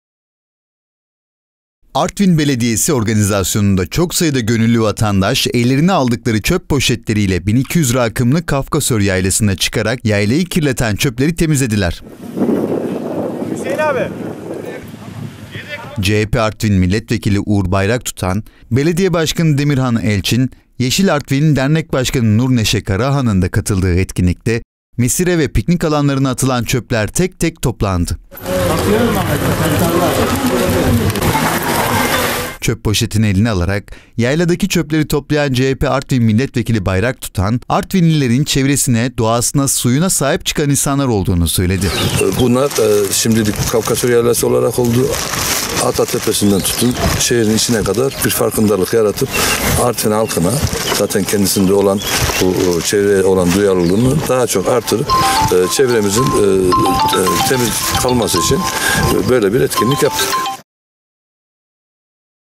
Bu kez boğa güreşi izlemek için değil, çöp toplamak için çıktılar
Her yıl geleneksel olarak boğa güreşleri festivalinin yapıldığı Kafkasör Yaylası'na Artvinliler bu kez atılan çöpleri toplamak için çıktı.

Artvin Belediyesi organizasyonunda çok sayıda gönüllü vatandaş ellerine aldıkları çöp poşetleriyle bin 200 rakımlı Kafkasör Yaylası'na çıkarak yaylayı kirleten çöpleri temizlediler.
CHP Artvin Milletvekili Uğur Bayraktutan, Belediye Başkanı Demirhan Elçin, Yeşil Artvin Dernek Başkanı Nur Neşe Karahan’ın da katıldığı etkinlikte mesire ve piknik alanlarına atılan çöpler tek tek toplandı.

Çöp poşetini eline alarak yayladaki çöpleri toplayan CHP Artvin Milletvekili Uğur Bayraktutan, “Artvinlilerin çevresine, doğasına, suyuna sahip çıkan insanlar olduğunu söyledi.

Artvin Belediye Başkanı Demirhan Elçin ise yaptığı açıklamada insanların doğaya karşı duyarlı olmaları için böyle bir farkındalık oluşturduklarını söyledi.